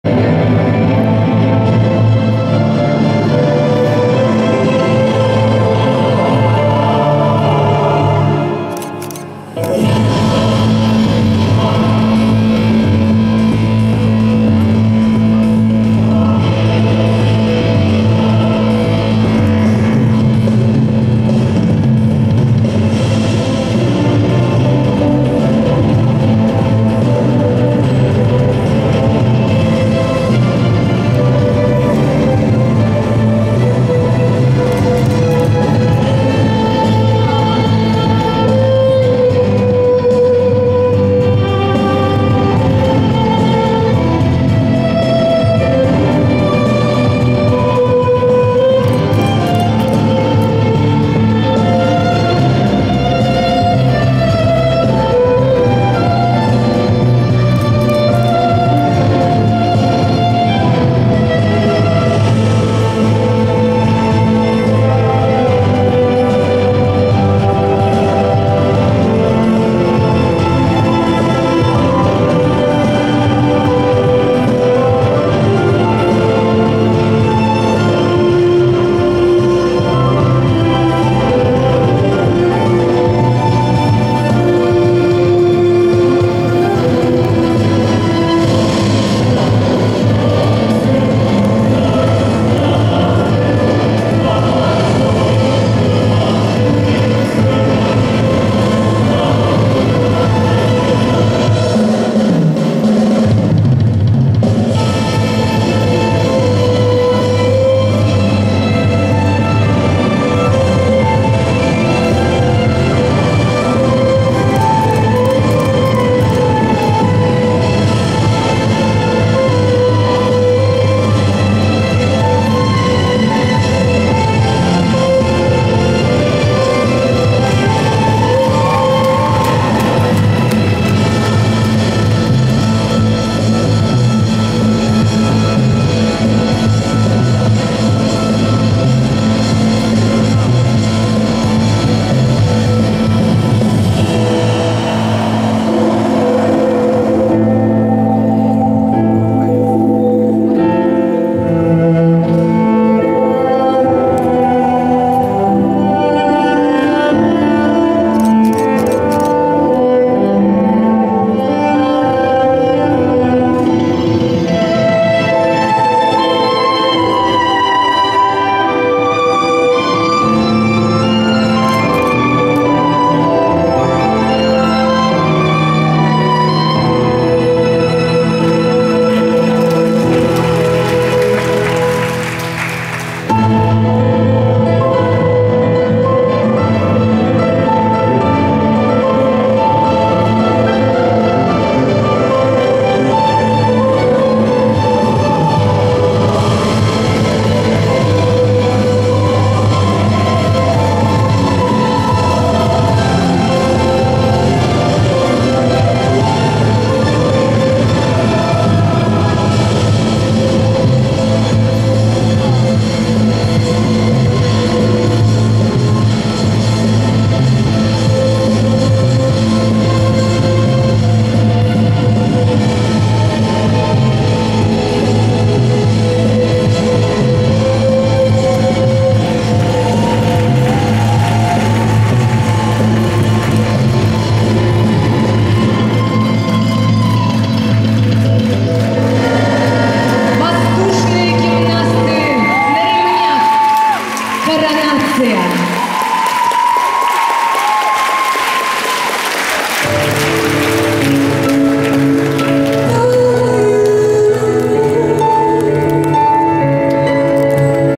Групповые ремни «Коронация» Святослав Савин, Эдуард Бозиян, Егор Новиков, Елизавета Ехлакова
Выпуск ГУЦЭИ - 2018. 13 июня